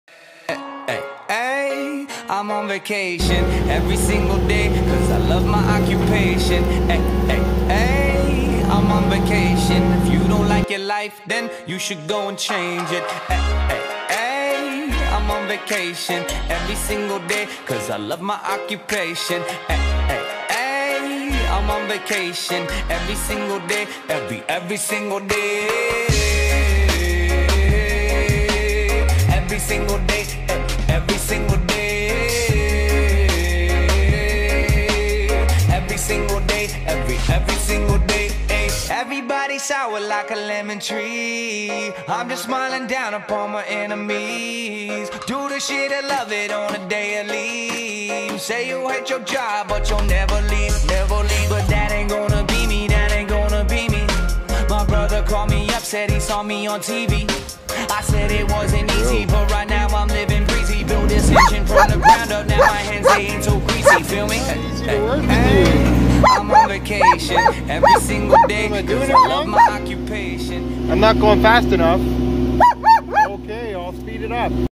Pond overflow system major upgrade using John Deere 17 excavator
A new 8-in overflow pipe was installed as well as an emergency spillway cut out.

the old system hat tree roots that had infiltrated the corrugated plastic culvert. the new system uses 8" DR-35 sanitary pipe and gasketed fittings, and will hold up for many years to come.

of course our 2 year warranty applies and will help ease the mind of the homeowner.